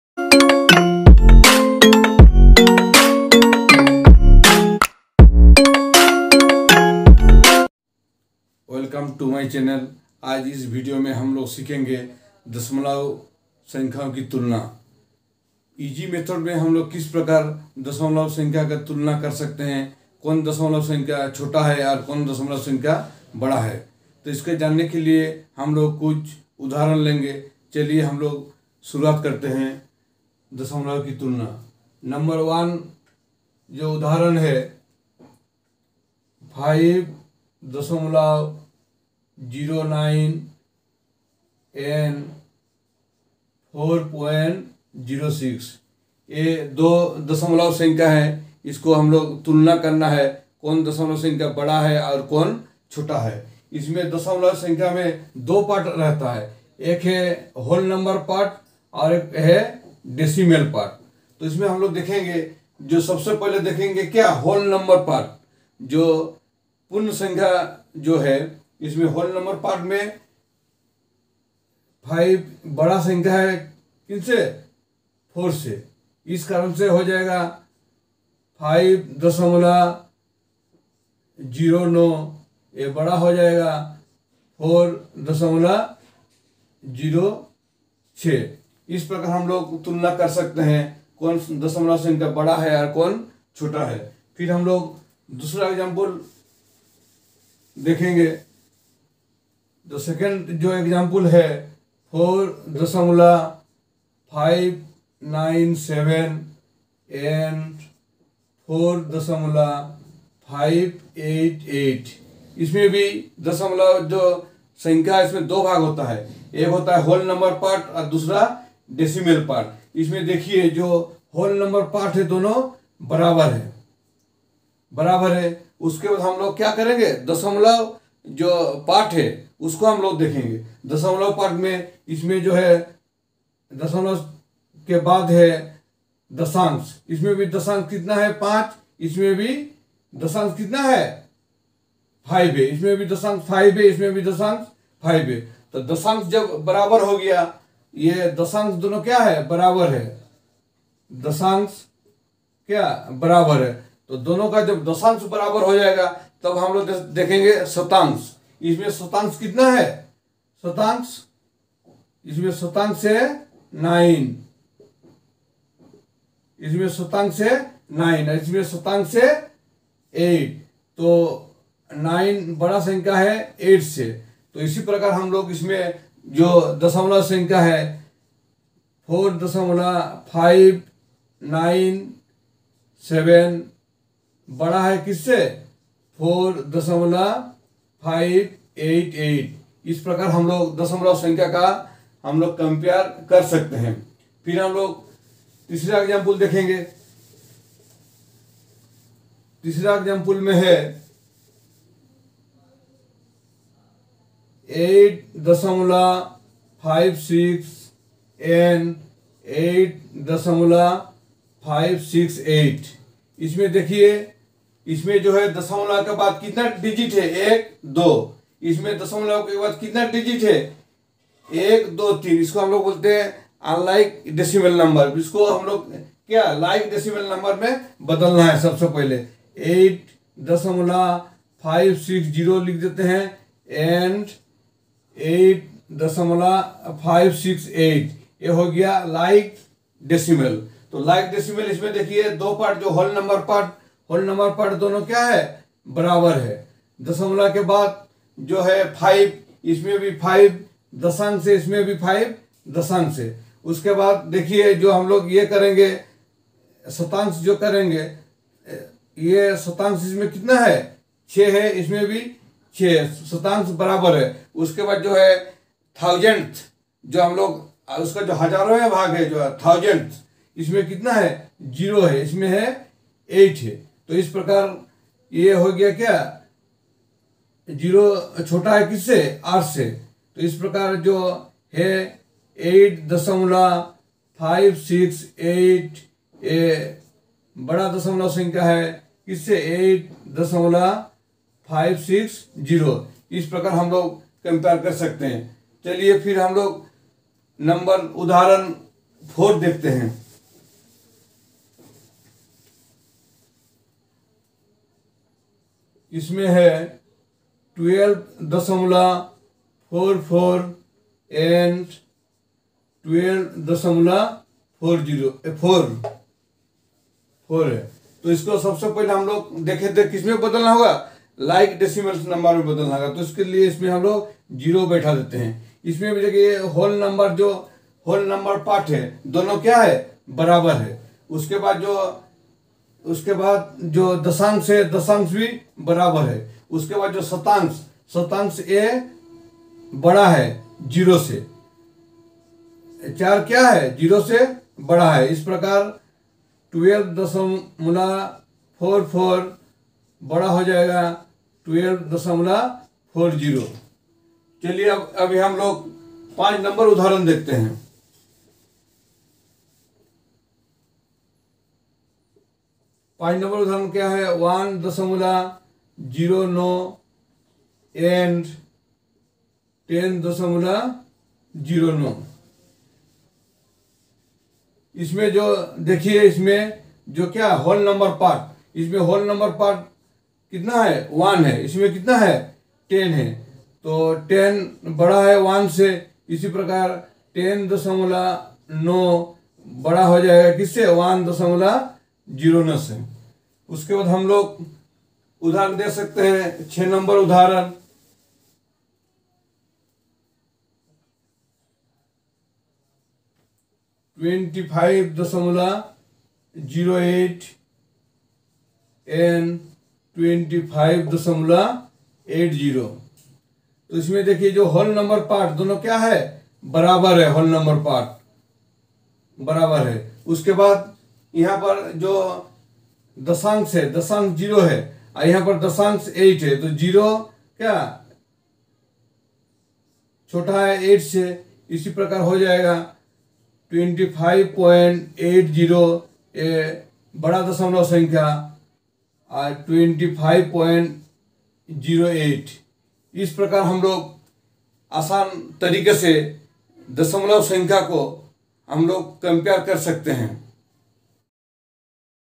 Comparing the Decimals | दशमलवों की तुलना | Class 6 and 7
This video is all about the comparing of Decimal
दशमलव की तुलना

If you have learn something from this video then